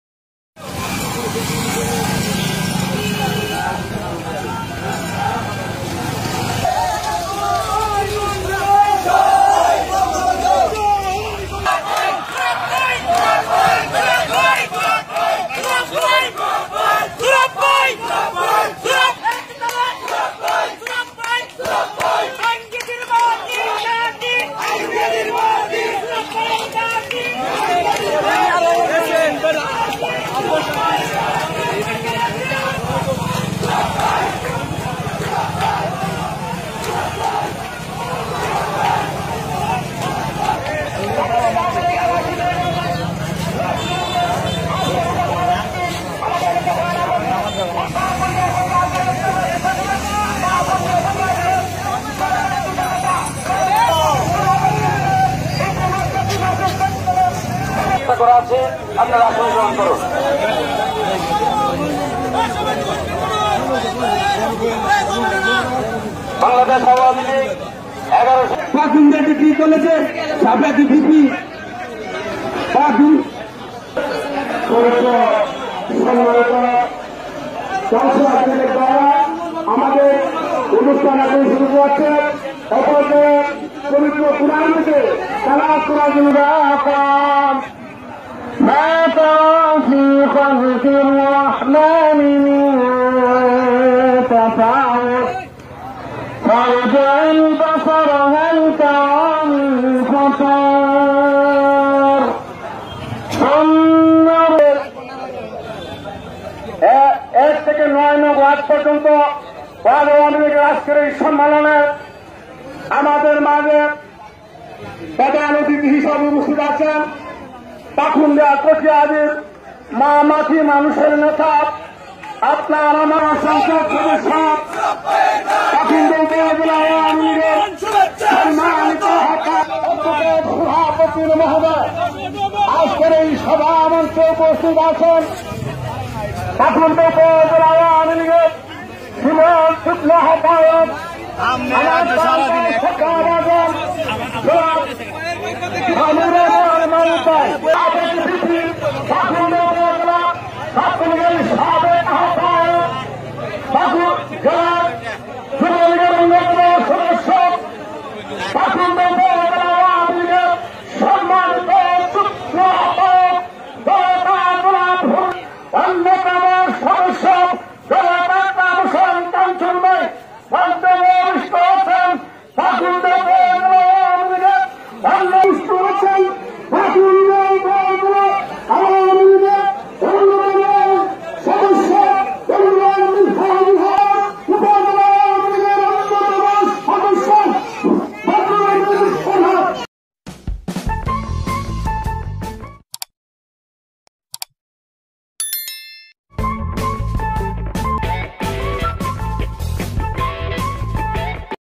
এগারসিন্দুর ইউনিয়ন আওয়ামী লীগের ত্রি-বার্ষিক সম্মেলন
এডভোকেট সোহরাব সামর্থকদের ইউনিয়ন সম্মেলন অনুষ্ঠিত